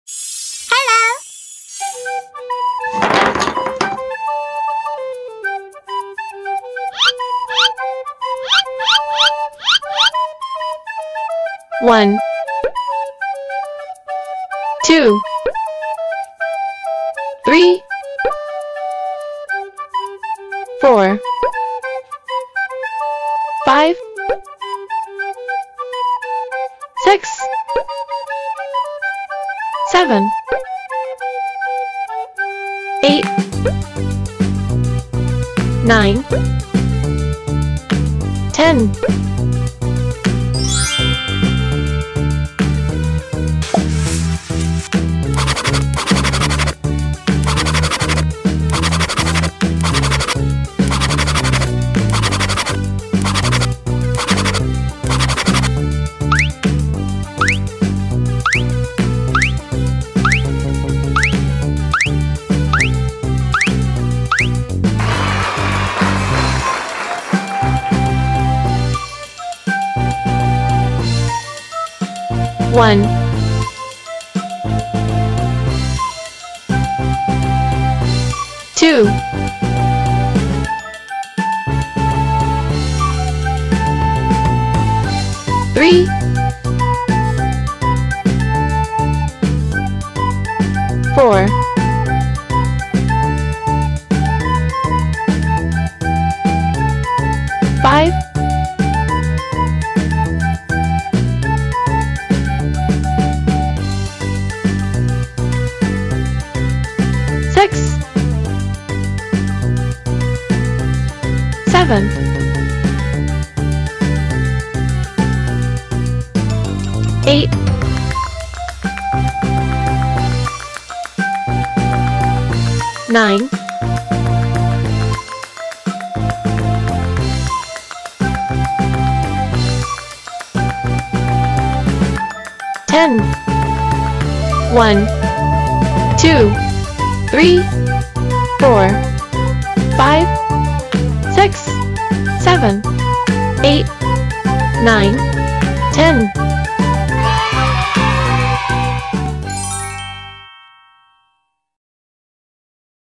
아이들을 위한 숫자 따라 그리기 색칠하기 | 숫자놀이
안녕하세요. ^^
Draw Sweet입니다.
아이들을 위한 숫자 그리기 색칠하기 모음 입니다.